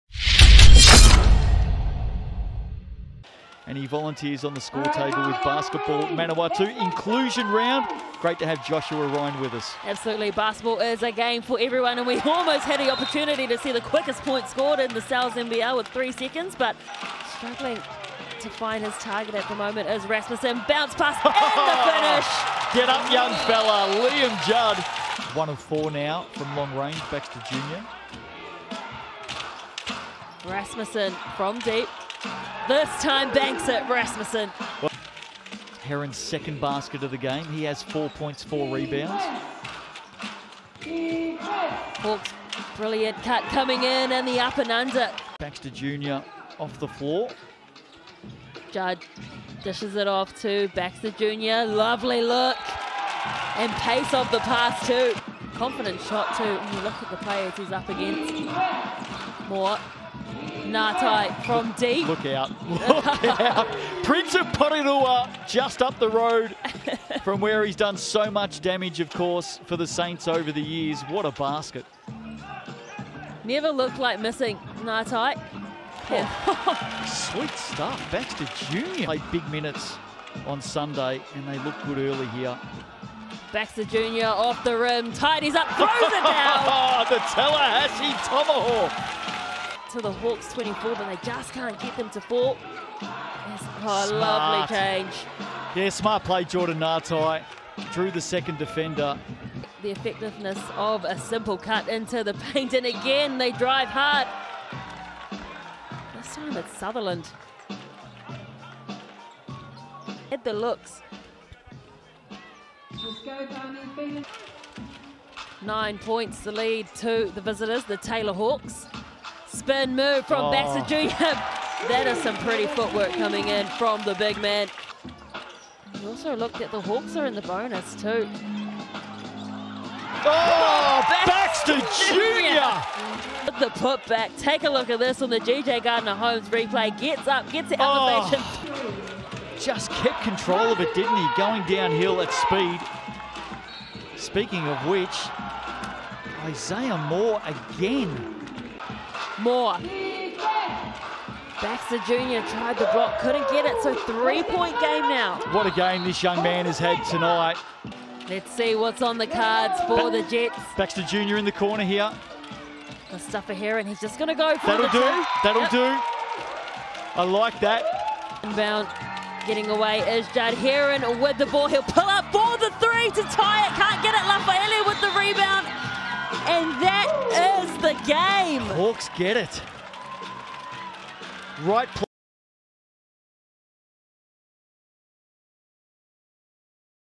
Manawatu Jets vs. Hawke’s Bay Hawks - Game Highlights
Watch the Game Highlights from Manawatu Jets vs. Hawke’s Bay Hawks, 04/11/2024